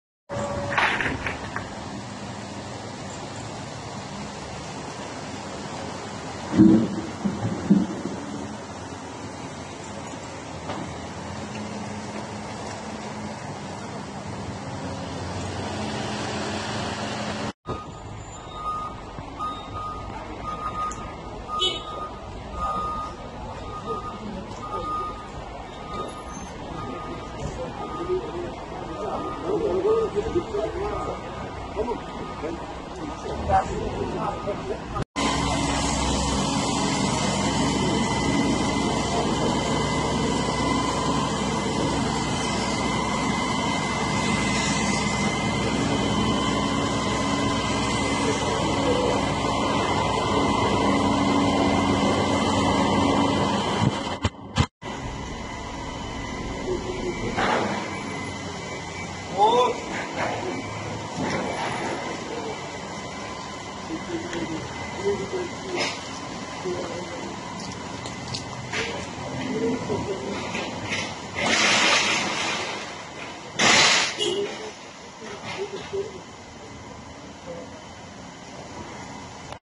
MUDANYA’DA SELİN YARALARI SARILIYOR
Mudanya Tirilye Mahallesi’nde şiddetli yağış sonrası oluşan sel hayatı adeta felç ederken, Valilik, Büyükşehir, BUSKİ ve AFAD ekipleri, yaşamın normale döndürülmesi için bölgede adeta seferberlik başlattı.